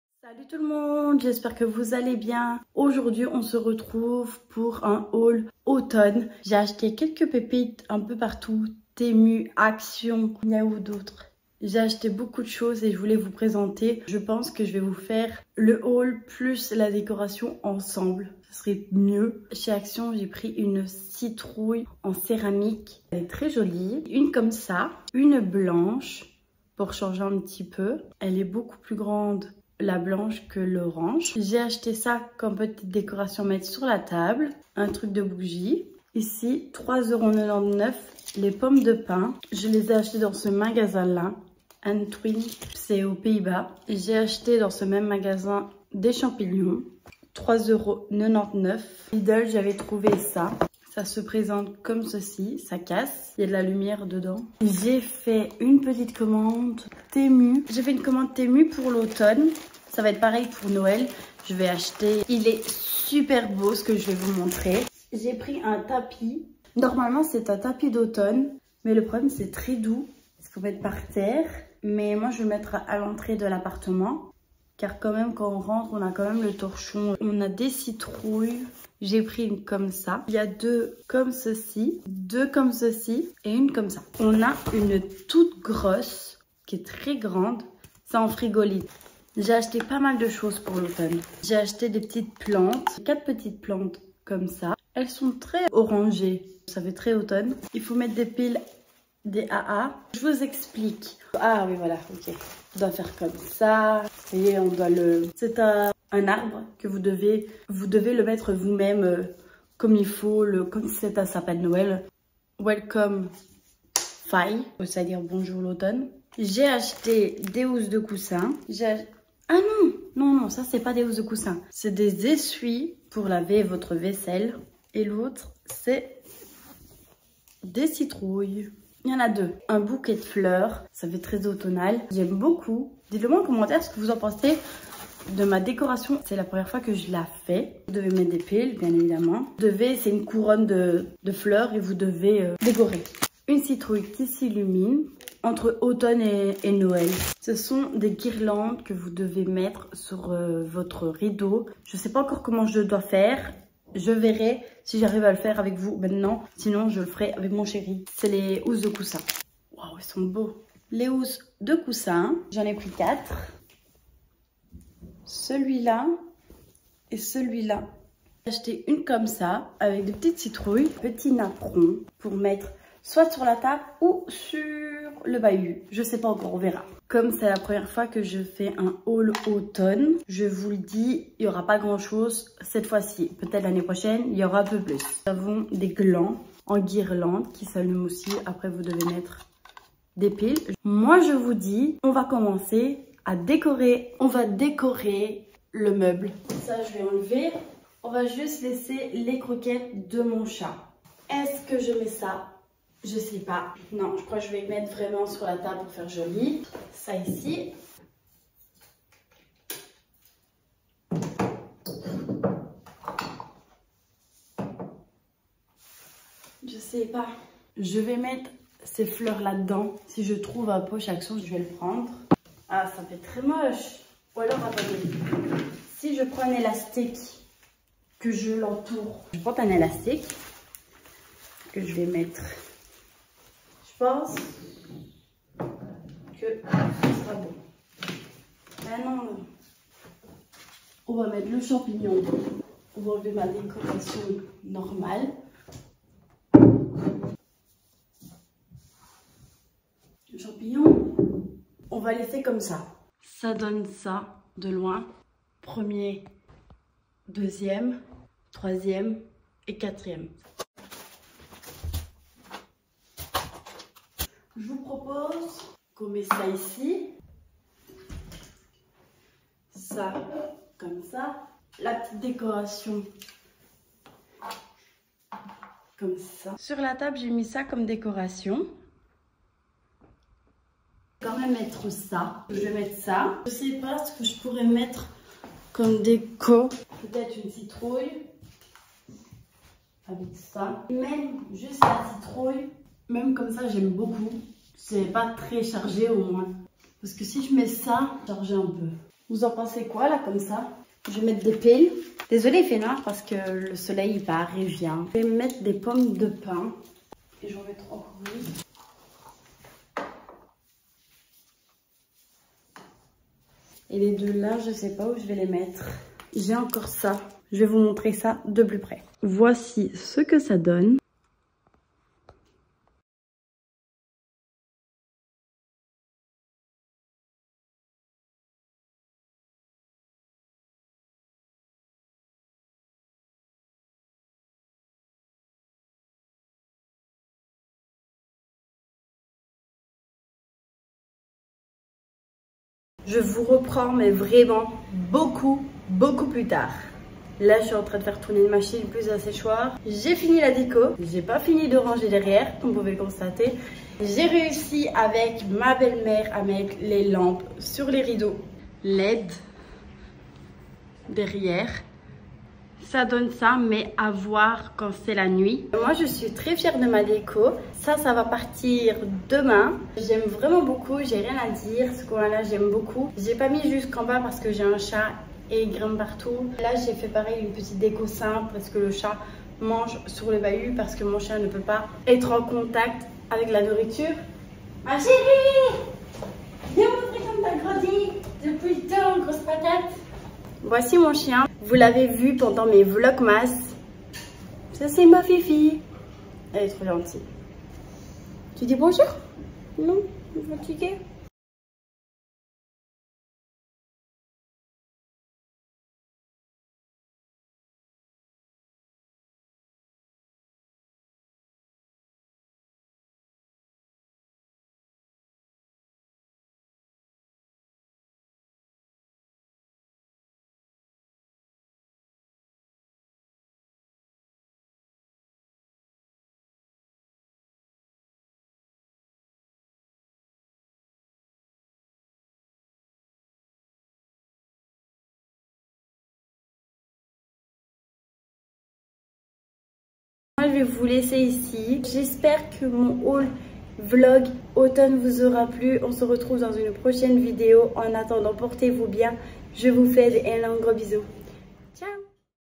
Haul spécial automne + on place les décorations 🍁
Salut tout le monde on se retrouve pour une nouvelle vidéo ça sera un haul  spécial automne ￼ je vous mets les liens en dessous de chaque article ￼

Il y a des liens que j’ai pas réussi à vous les mettre car ils sont plus disponibles ￼,c’était pour vous dire aussi que c’était la dernière vidéo et on se retrouve pour les￼ vlogmas le 2 decembre